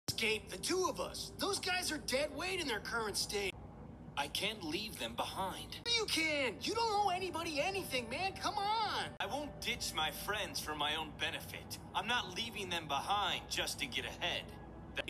Yugioh rest in paradise kazuki takahashi
Ii Made a video dedicated to him yugioh carda Yugioh master duel may the king of games rest in paradise Kazuki takahashi short video japan Japanese anime